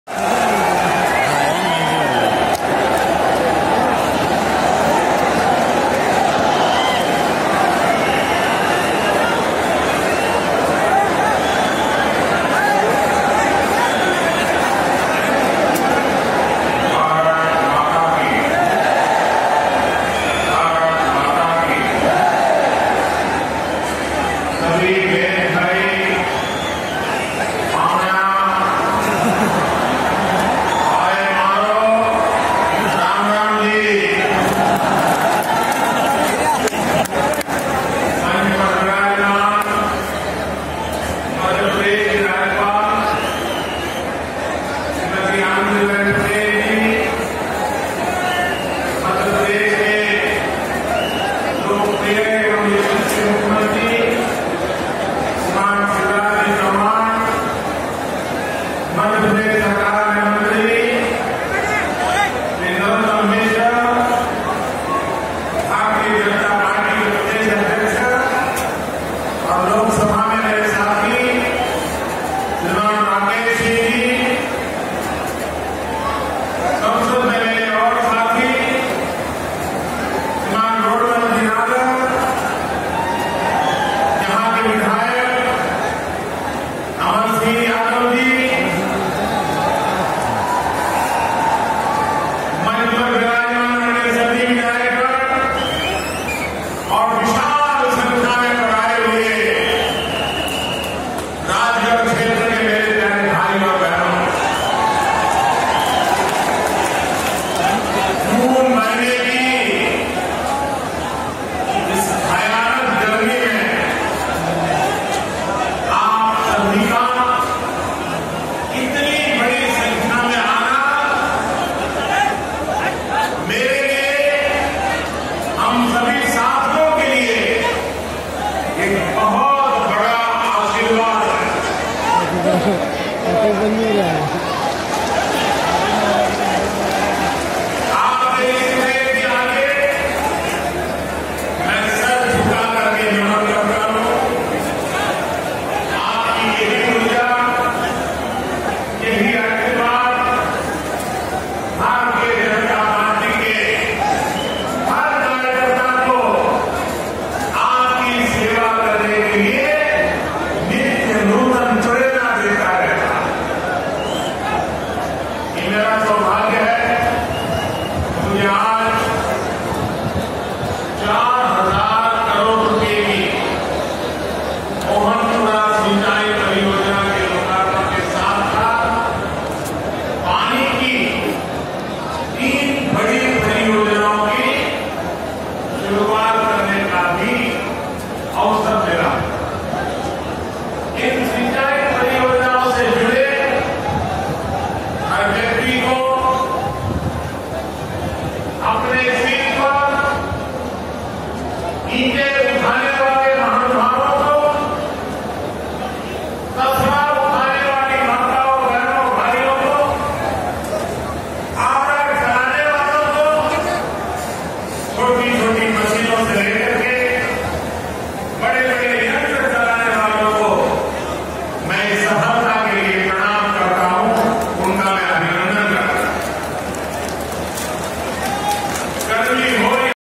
राजगढ़ प्रधानमंत्री नरेंद्र मोदी जी एवं मुख्यमंत्री शिवराज सिंह जी ने किया मोहनपुरा बांध का लोकार्पण
Mohanpura Dam Project Inauguration By Primeminister Shree Narendra Modi Ji and Chief Minister M.P Shree Shivraj Singh Chouhan .